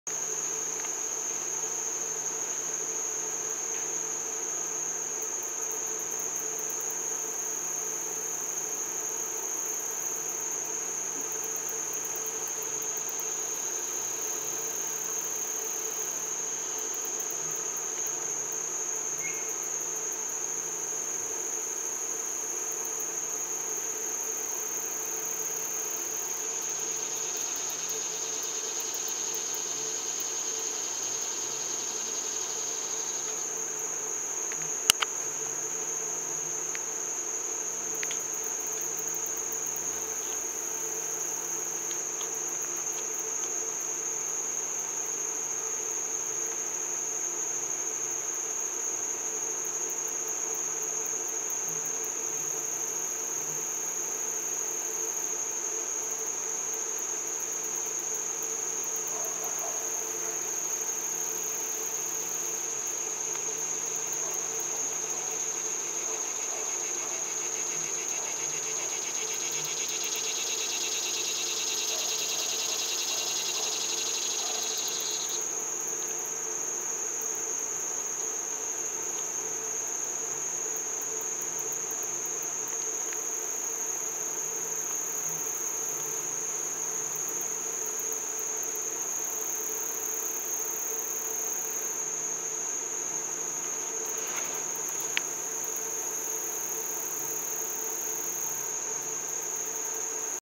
Ahsa the Red Panda
Someone has come a long way since I first saw her getting her butt cleaned.

Also, points if you found Neo in this video before I give it away. Of course the points are all made up and mead absolutely nothing. Now its time for scenes from a hat.

Ahsa {female} and Neo {male}
Greenville Zoo
Greenville, South Carolina